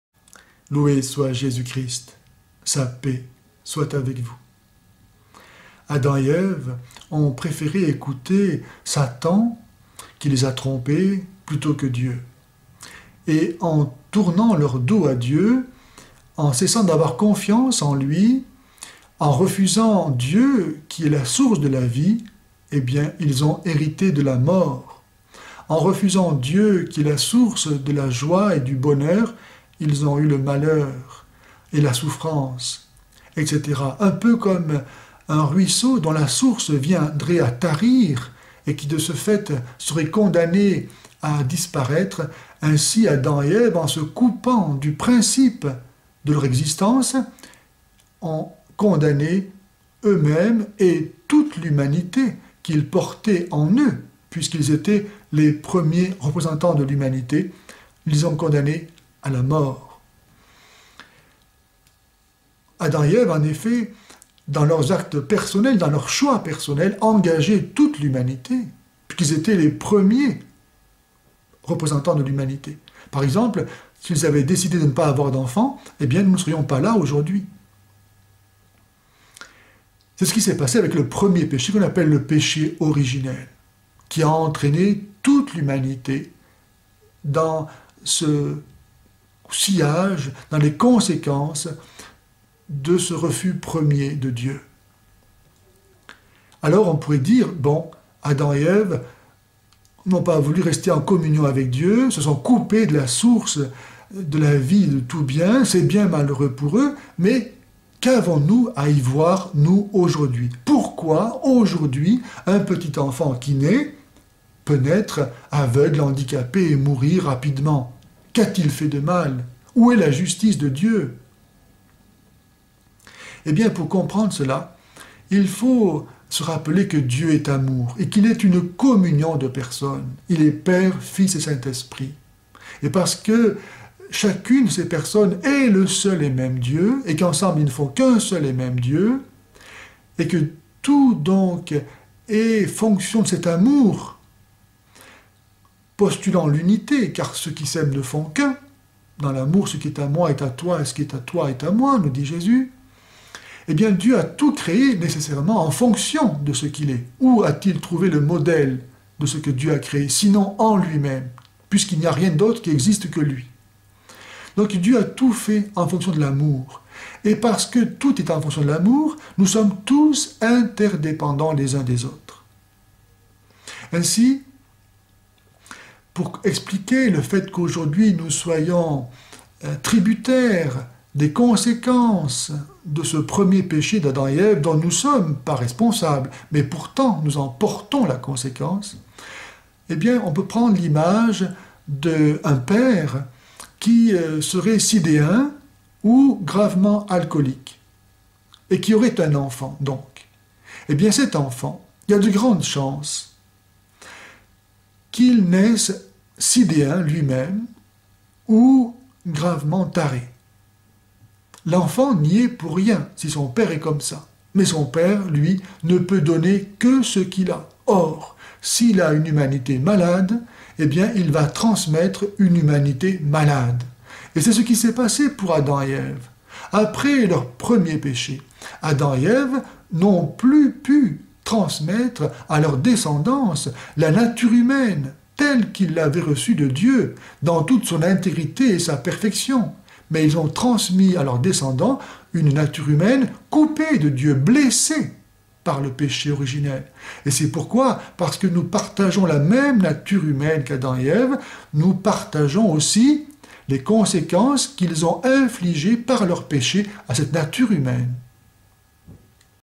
pourquoi le mal ou le péché originel 4 sur 4 avec sous-titre fr disponible et transcription.
Loué soit Jésus-Christ, Sa paix soit avec vous ! Adam et Ève ont préféré écouter Satan qui les a trompé... plutôt que Dieu.

Et en tournant leur dos à Dieu, en cessant d'avoir confiance en Lui, en refusant Dieu qui est la source de la vie,

eh bien, ils ont hérité de la mort. En refusant Dieu qui est la source de la joie et du bonheur, ils ont eu le malheur

et la souffrance etc. Un peu comme un ruisseau dont la source viendrait à tarir, et qui de ce fait

serait condamné à disparaître. Ainsi Adam et Ève, en se coupant du principe de leur existence ont

condamné eux-mêmes et toute l'humanité... qu'ils portaient en eux, puisqu'ils étaient les premiers représentants

de l'humanité; ils ont condamné à la mort. Adam et Ève en effet, dans leurs actes personnels, dans leurs choix personnels,

engageaient toute l'humanité parce qu'ils étaient les premiers représentants de l'humanité. Par exemple:

s'ils avaient décidés de ne pas avoir d'enfants, eh bien, nous ne serions pas là aujourd'hui. C'est ce qui s'est passé

 avec le premier péché que l'on appelle le "péché orginel". Qui a entraîné toute l'humanité dans

ce sciage, dans les conséquences de ce refus premier de Dieu.

 Alors, on pourrait dire: "Bon, Adam et Ève n'ont pas voulu rester en communion avec Dieu, se sont coupés

de la source de la vie de tout bien, c'est bien malheureux pour eux:

 "Mais qu'avons-nous à y voir nous aujourd'hui?" Pourquoi aujourd'hui, un petit enfant qui né peut naître:

 aveugle, handicapé et mourir rapidement? Qu'a-t-il fait de mal? Où est la justice de Dieu?

Eh bien pour comprendre cela, il faut se rappeler que Dieu est amour et qu'Il est une communion de personnes.

Il est Père, Fils et Saint-Esprit!!! Et parce que chacune de ces personnes est le seul et même Dieu,

et qu'ensemble ils ne font qu'1 seul et même Dieu, et que tout donc est fonction de cet amour

postulant l'unité: car ceux qui s'aiment ne font qu'un. Dans l'amour, ce qui est à moi est à toi et ce qui

est à toi est à moi nous dit Jésus, Eh bien, Dieu a tout créé nécessairement en fonction de ce qu'Il est.

 "Où a-t-Il trouvé le modèle de ce que Dieu a créé?" Sinon en Lui-même. Puisqu'il n'y a rien d'autre

qui existe que Lui. Donc, Dieu a tout fait en fonction de l'amour. Et parce que tout est en fonction de l'amour,

nous sommes tous interdépendants les uns des autres. Ainsi, pour expliquer le fait qu'aujourd'hui

nous soyons tributaires des conséquences de ce premier péché d'Adam et Ève, dont nous sommes pas responsable

 mais pourtant nous en portons la conséquence. Eh bien, on peut prendre l'image d'un père qui serait sidéen

 ou gravement alcoolique, et qui aurait un enfant donc. Eh bien cette enfant, il y a de grandes chances

qu'il naisse sidéen lui-même, ou gravement taré. L'enfant n'y est pour rien si son père est comme ça.

Mais son père lui ne peut donner que ce qu'il a. Or, si il a une humanité malade, eh bien il va transmettre

 une humanité malade. Et c'est ce qui s'est passé pour Adam et Ève. Après leur premier péché, Adam et Ève

n'ont plus pu transmettre à leur descendance la nature humaine telle qu'ils l'avaient reçu de Dieu,

dans tout son intégrité et sa perfection. Mais ils ont transmis à leurs descendants une nature humaine

coupée de Dieu, blessée.. par le péché originel. Et c'est pourquoi parce que nous partageons la même

nature humaine qu'Adam et Ève, nous partageons aussi les conséquences qu'ils ont infligé par leur péché

à cette nature humaine.